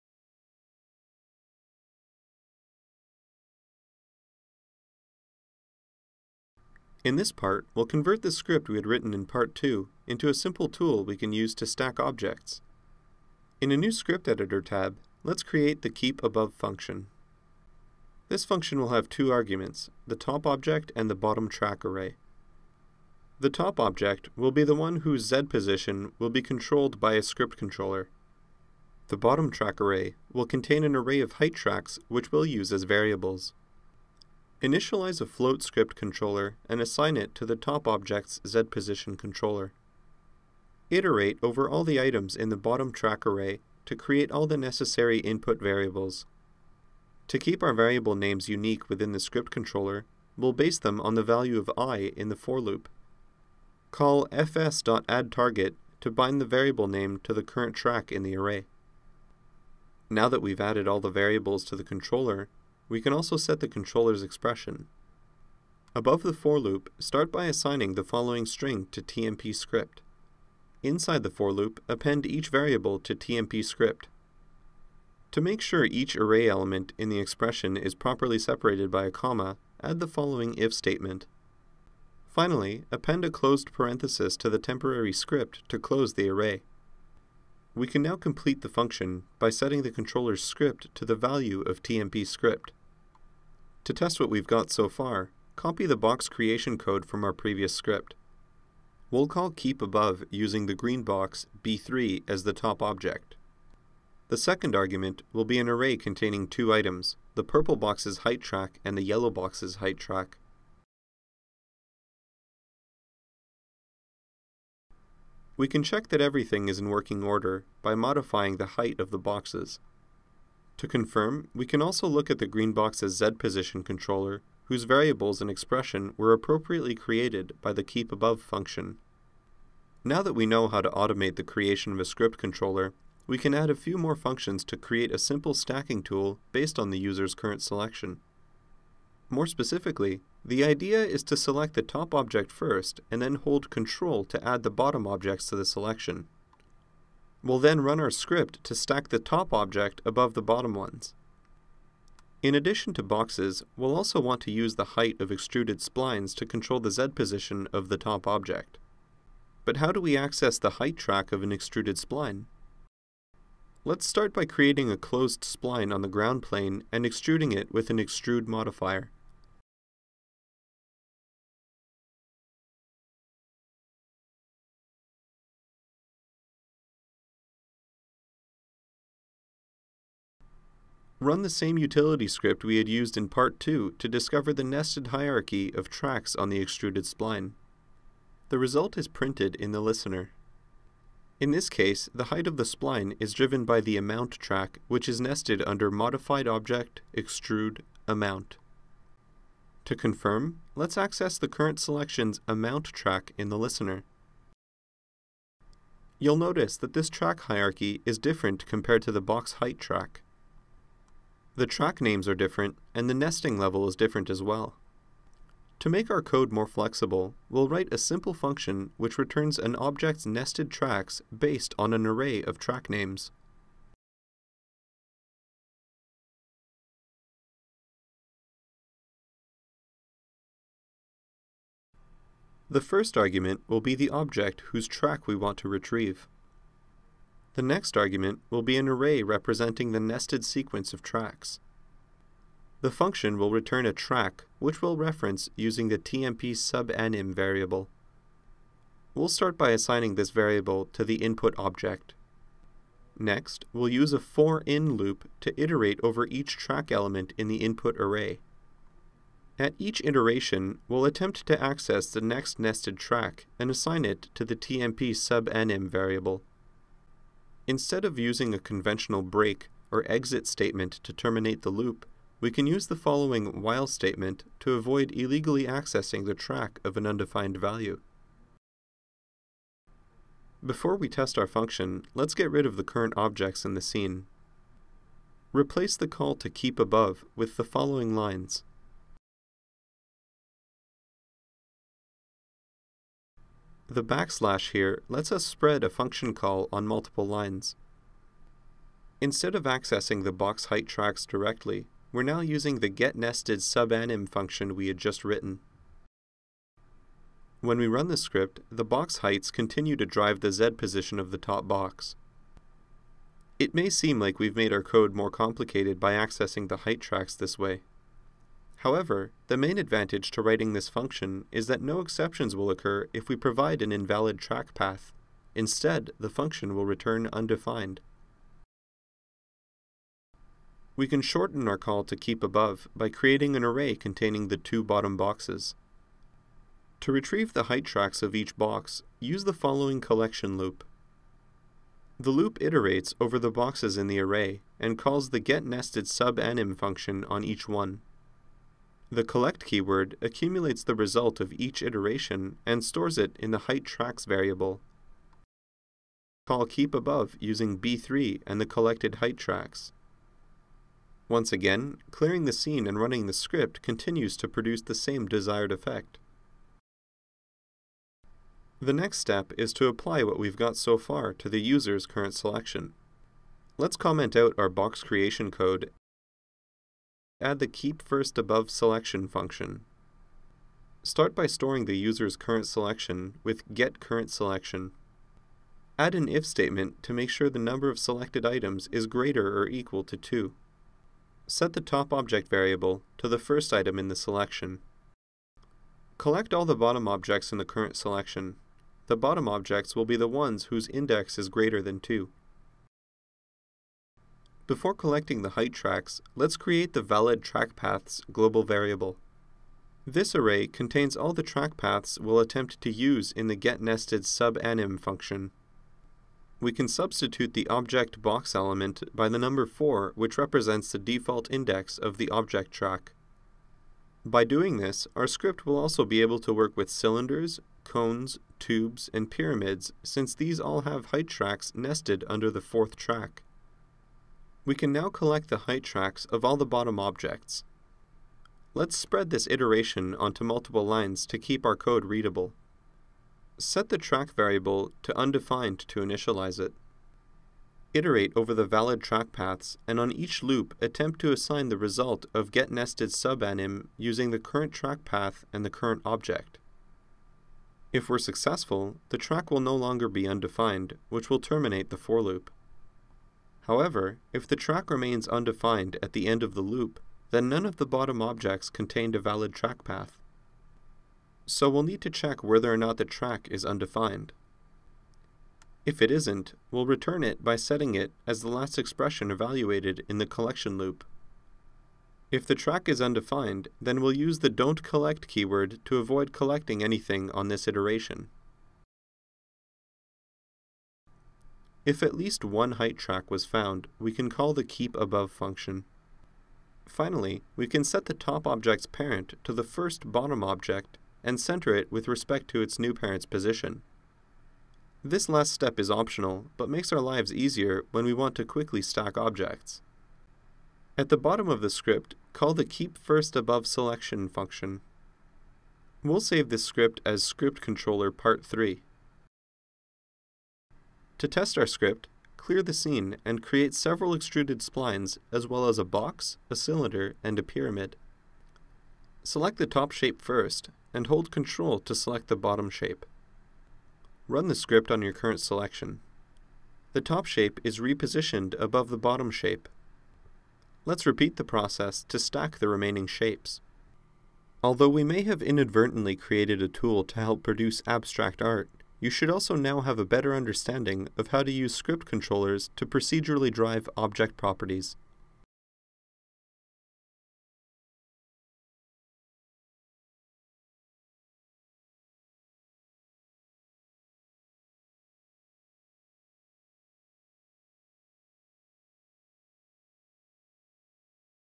Script Controllers - Part 3: Using Script Controllers to Create a Stacking Tool
In this movie, we further automate the creation of Script Controllers using MaxScript. We also show how to access the user's current selection to create a simple shape stacking tool.